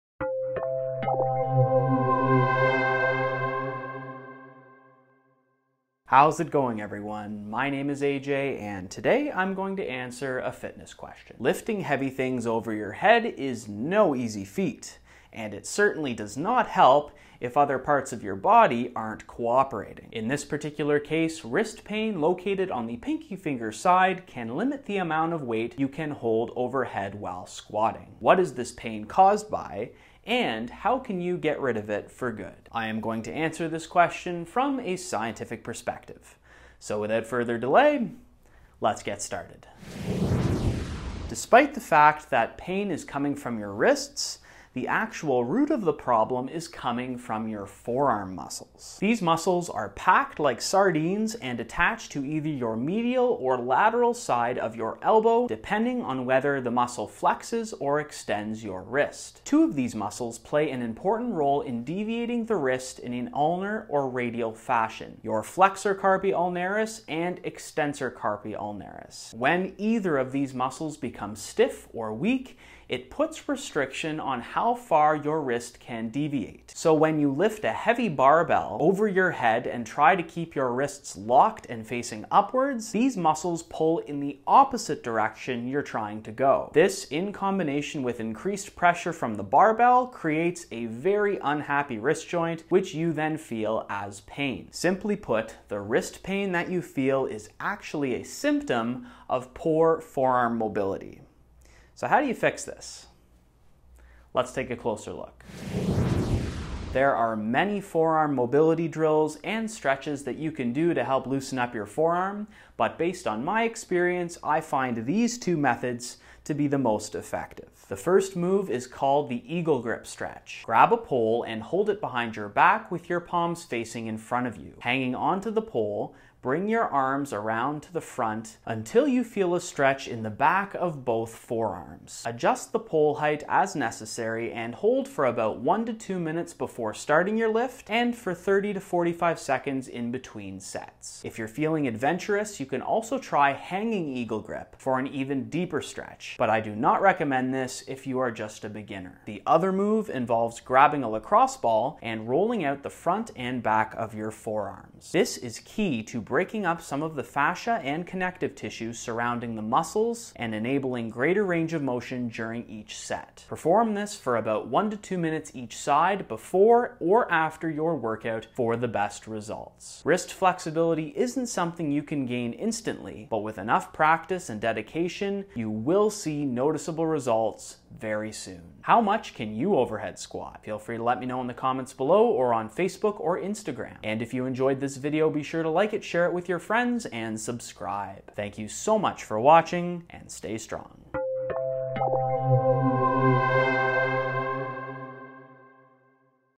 Why Does The Pinky Side Of My Wrist Hurt During Overhead Squats & How Do I Fix This?
Lifting heavy things is no easy feat, and it certainly doesn’t help when other parts of your body aren’t cooperating. In this particular case, wrist pain located on the pinky finger side can limit the amount of weight you can hold overhead while squatting which in turn will limit your gains. What is this pain caused by, and how do you get rid of it for good? A.J. answers this questions from a scientific perspective.